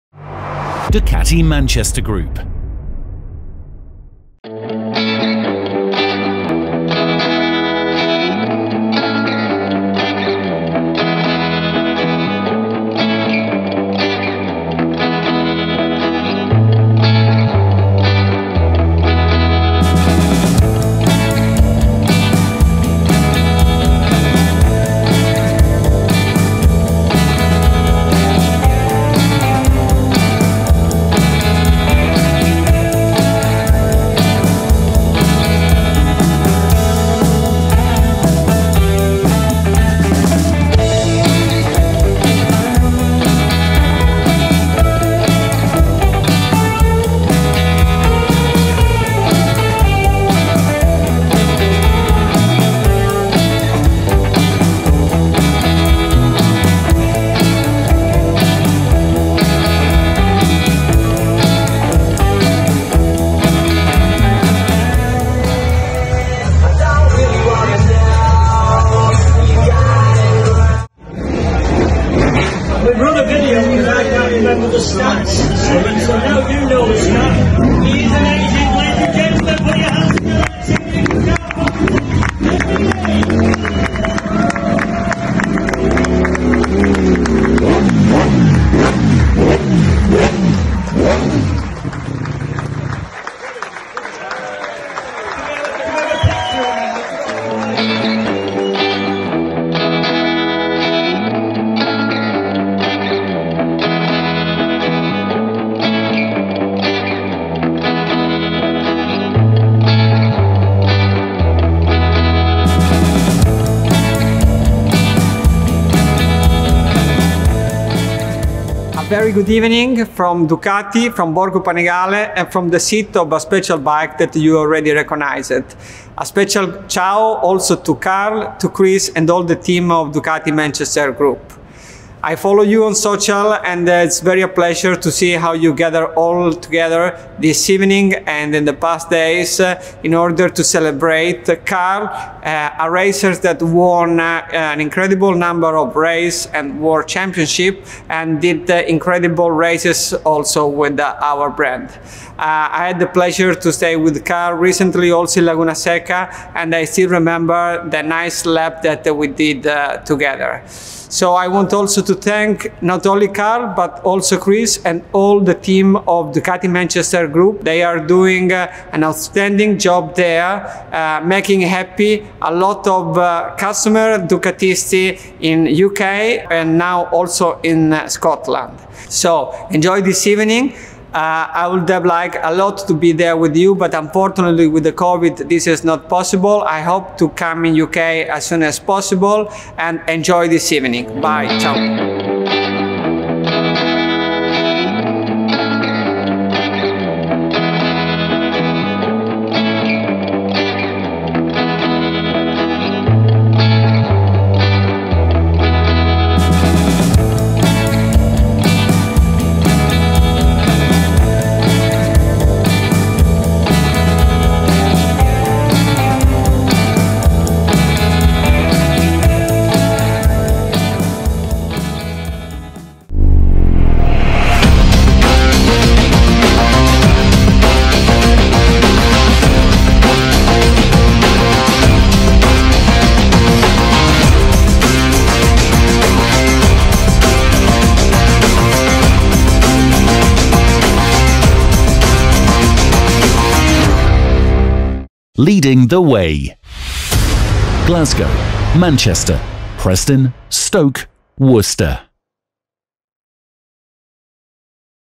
Foggy Chat Show
An Evening with Foggy is a Fun Evening. This event for Ducati Manchester Group at The Crofters in Bolton was a hit with the punters